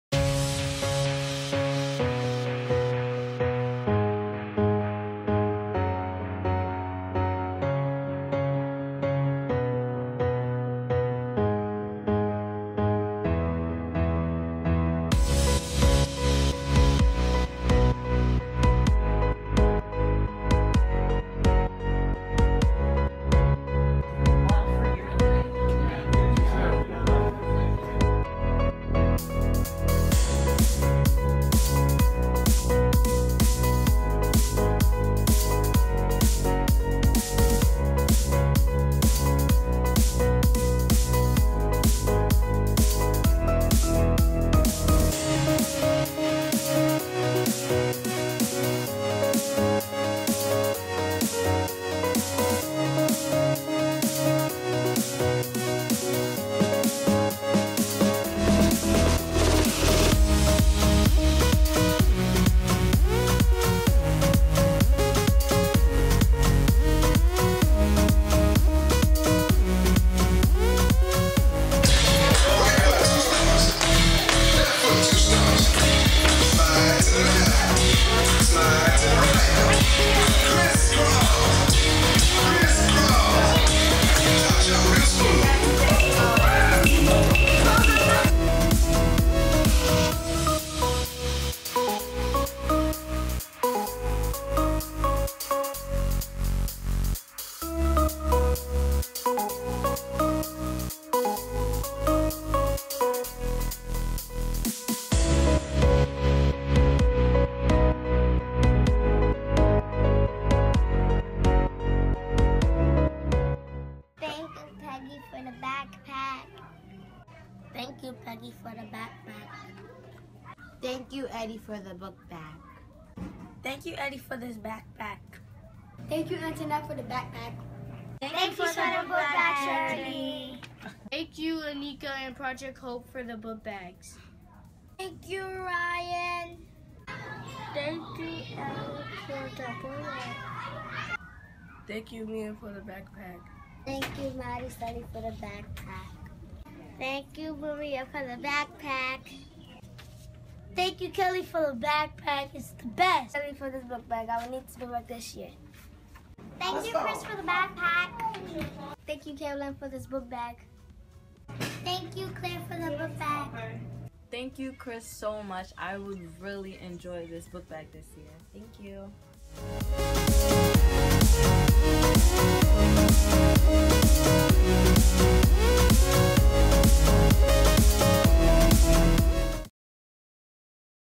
Dare2B Backpack Distribution Event 2015
After two months of campaigning the Sponsor a Child Backpack Program, Radio 103.9FM joined the Dare2B team to distribute the donated backpacks to homeless children in the Bronx.  Radio Director Raphael along with Radio DJ Roxie and their street team leader John enhanced the children's experience by bringing the music beats, games and some really cool prizes. Thank you Radio 103.9FM for helping us raise awareness on the different face of homelessness and brightening the lives of hundreds of children.  Special thanks to our amazing sponsors who made this event possible.  For complete photos visit our facebook page using the Dare2BNYC handle.